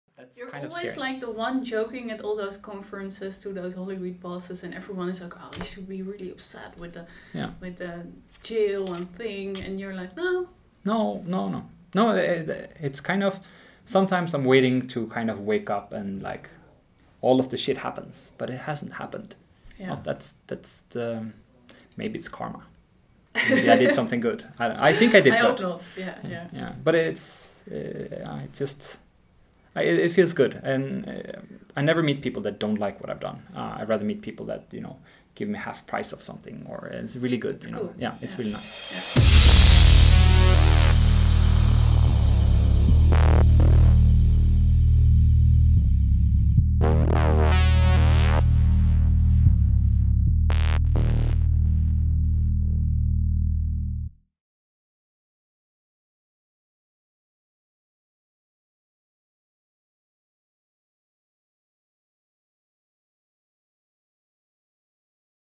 Peter Sunde: 'I am relaxed about the whole jail thing'
Peter Sunde, founder of the Piratebay and Flattr, was arrested just two months after this interview.

This is the tenth interview in the quest for Future Fuel, the power of young people as fuel for the future. Anne Walraven makes a journey across four continents to interview 15 of the most inspiring doers and thinkers of our time about the role of the young generation within the enormous challenges we face. Or: can someone please tell me how the world works?